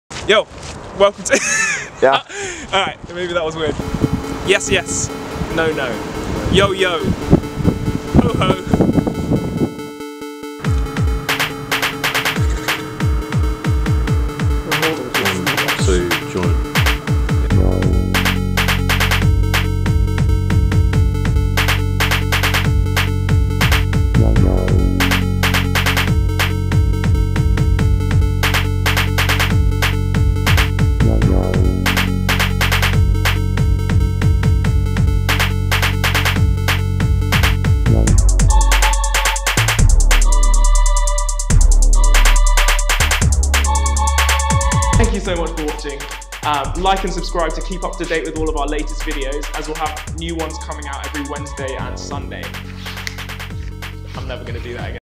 What is Tell Me About?
Tell Me About brings you informative discussions with leading experts.

Releasing new videos on Wednesdays and Sundays.